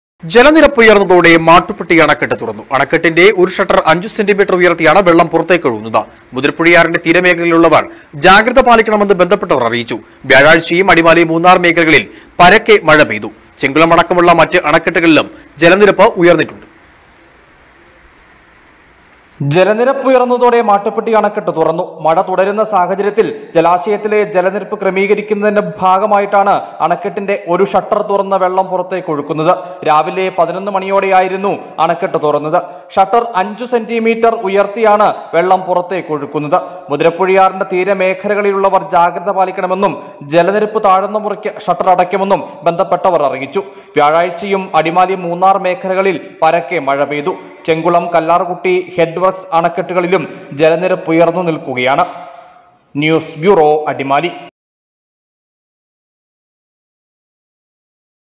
മാട്ടുപ്പെട്ടി അണക്കെട്ട് തുറന്നു. അണക്കെട്ടിന്റെ ഒരു ഷട്ടര്‍ 5 സെന്റിമീറ്റര്‍ ഉയര്‍ത്തി
മുതിരപ്പുഴയാറിന്റെ തീരമേഖലകളില്‍ ഉള്ളവര്‍ ജാഗ്രത പാലിക്കണമെന്ന് ബന്ധപ്പെട്ടവര്‍ അറിയിച്ചു.l